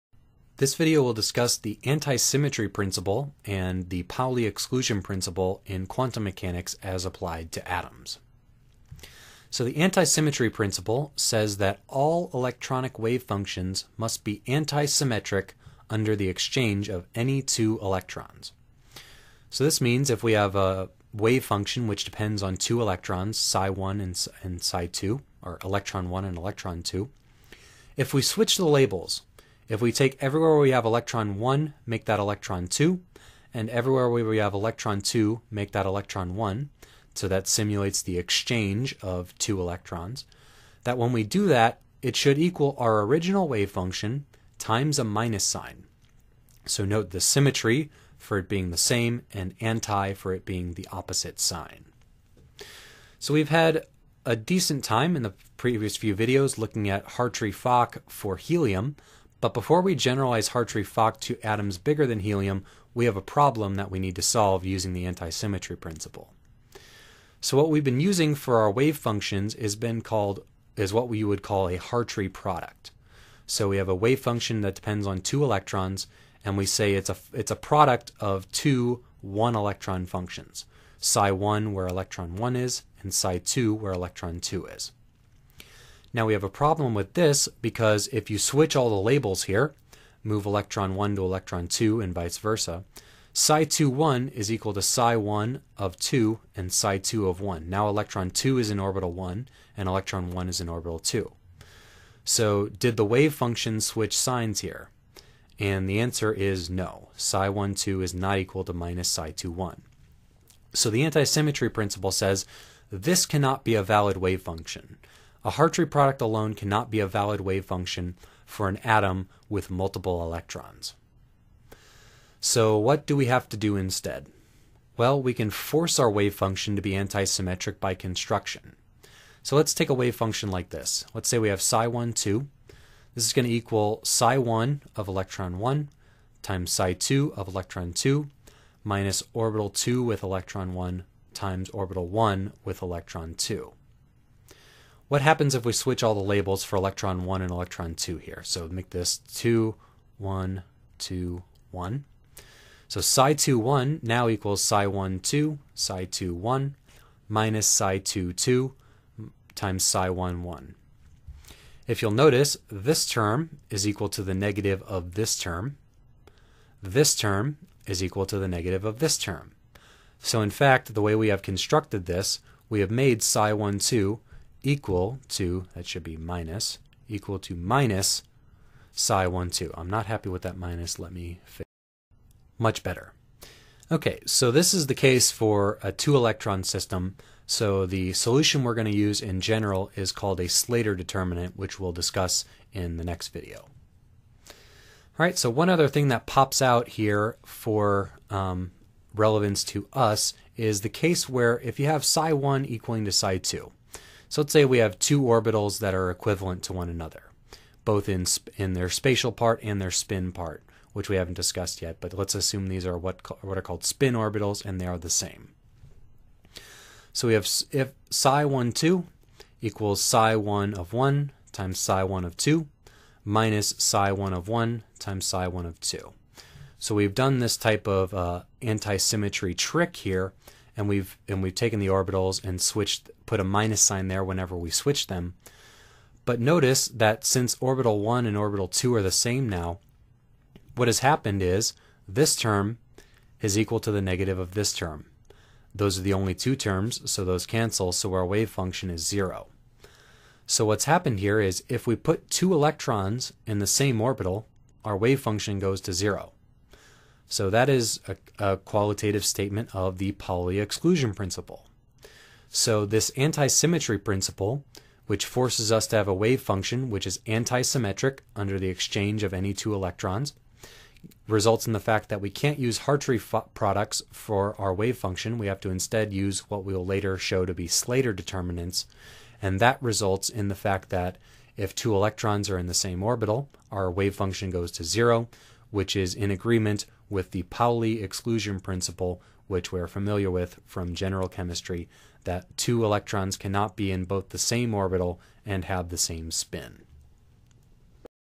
Quantum Chemistry 9.5 - Antisymmetry Principle
Short lecture on the antisymmetry principle for electrons.

The wavefunction of any atom or molecule must switch sign when two electrons are exchanged. This anti-symmetry principle results in a total wavefunction which is a Slater determinant. This is the physical origin of the Pauli exclusion principle, which makes the wavefunction go to zero when two electrons are placed in the same spin orbital.

--- Video Links ---

--- Social Links ---

--- Equipment ---

Drawing Tablet: Wacom Intuos Pen and Touch Small

Drawing Program: Autodesk Sketchbook Express